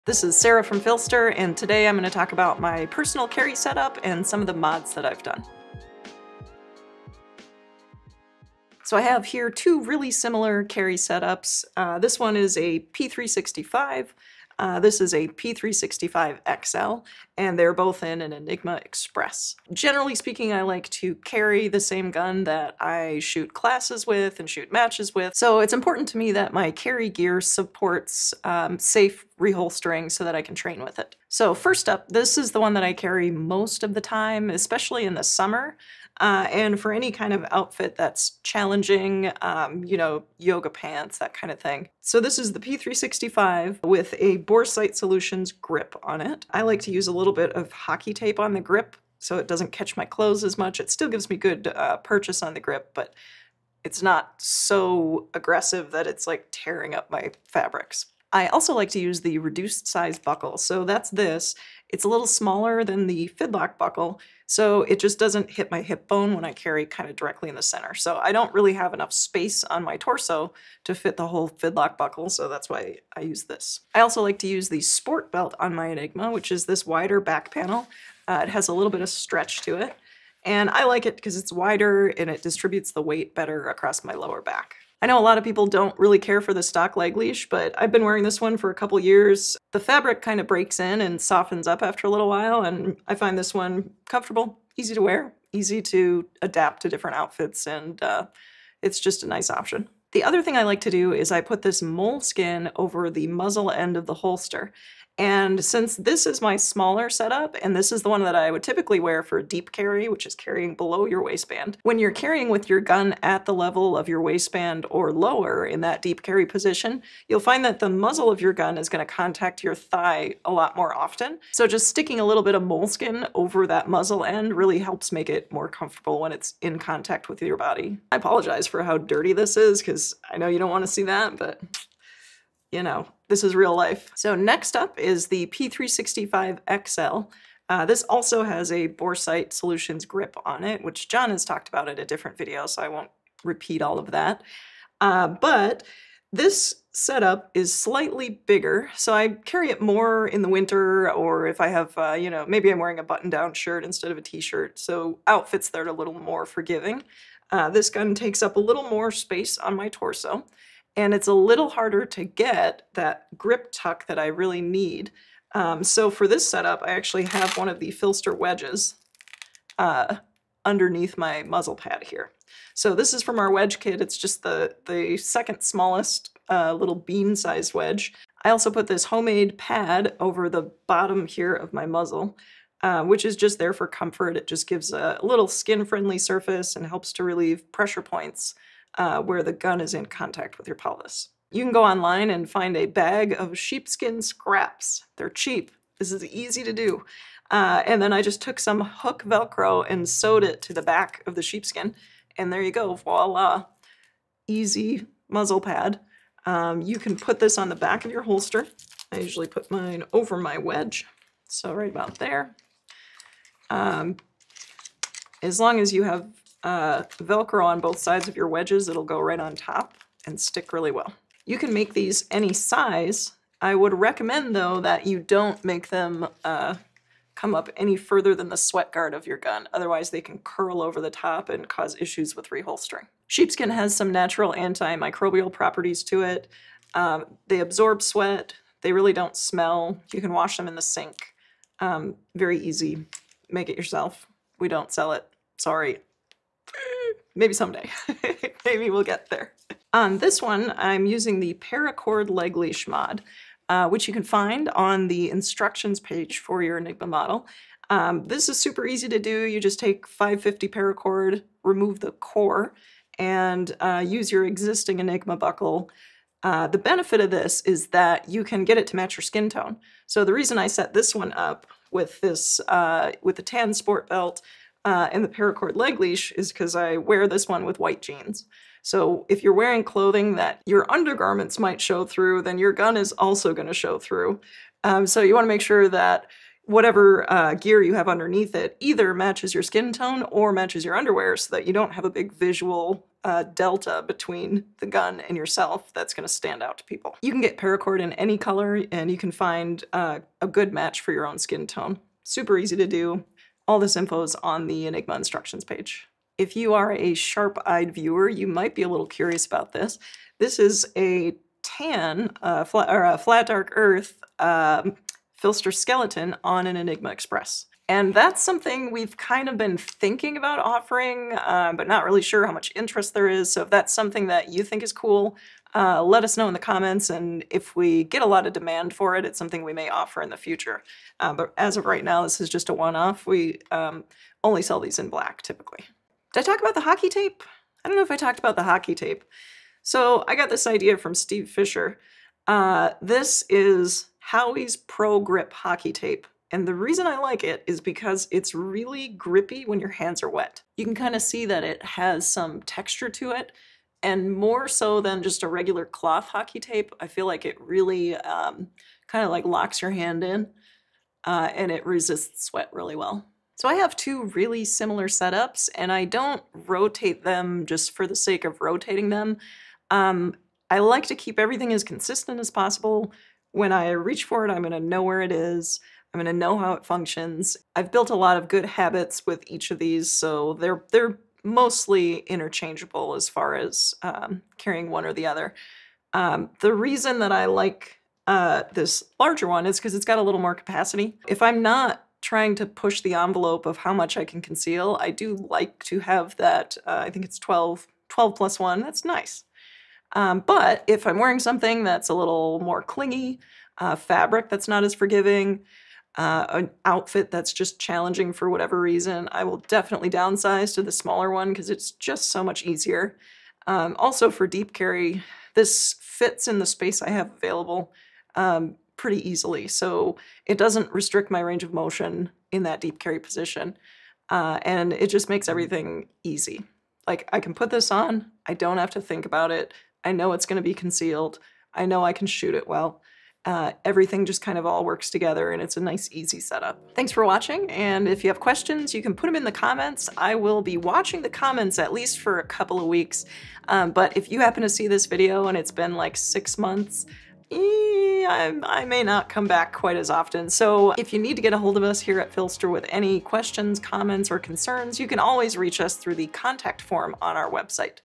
Sarah's P365 + P365XL Customized Enigma Express
In this video Sarah shares how she customized her P365, and her P365XL Enigmas.

Chapters:
0:00 Intro
0:30 Sig P365
0:57 Enigma customizations
1:51 Deep carry
2:14 Sig P365XL
2:47 Customizations
3:11 Sheepskin muzzle pad
4:11 Nude leg leash
5:08 FDE Skeleton
5:36 Hockey tape
6:00 Sig P365 + P365XL

We stand by to assist you with any questions you may have about the product, and provide any assistance you may need.